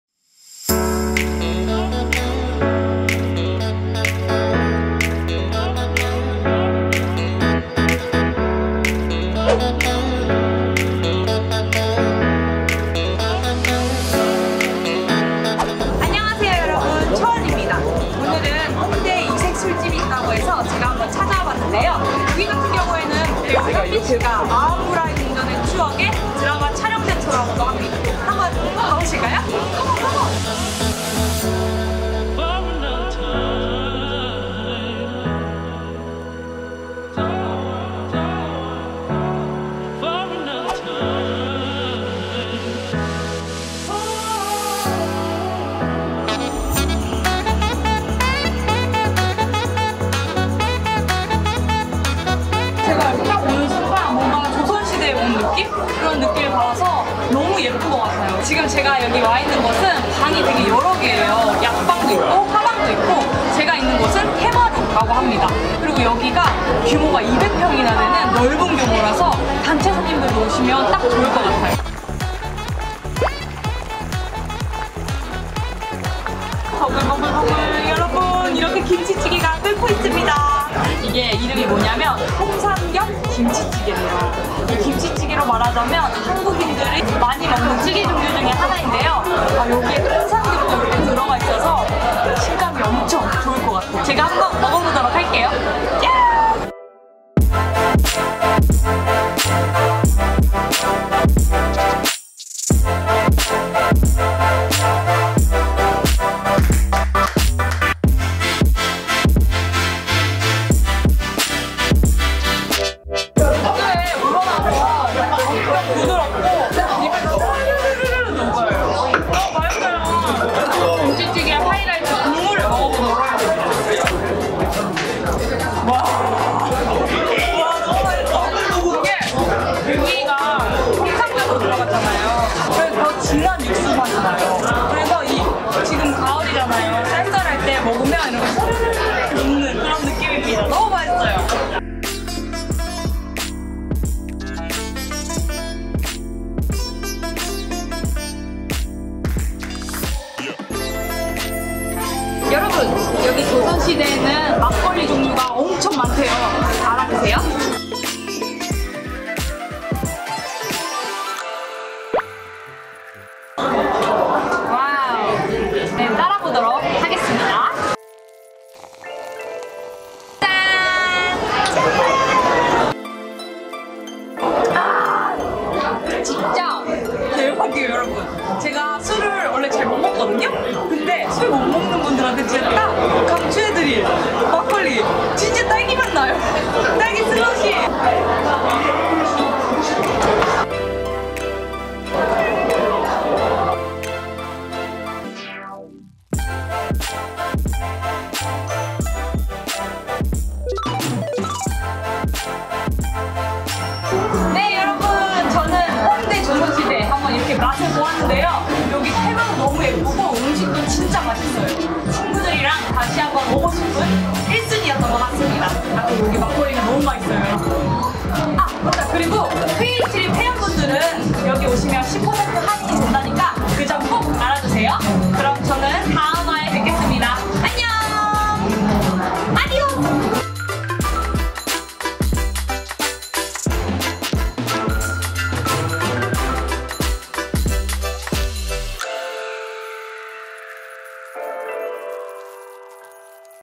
【韓國美食】 來弘大一定要去的馬格里酒館 (Feat. 免費租借韓服) (中文 SUB)｜Creatrip
來弘大一定要來的特別馬格里酒館介紹 ! 🍶
這裡還可以租借韓服，不來太可惜了！ 😙✨
🎉不要錯過Creatrip的會員折扣！

朝鮮時代
營業時間: 每天 00:00 ~ 24:00
住址: 44 B1 of Hongik-ro 3-gil, Mapo-gu, Seoul
서울 마포구 홍익로3길 44 지하1층

追蹤我們!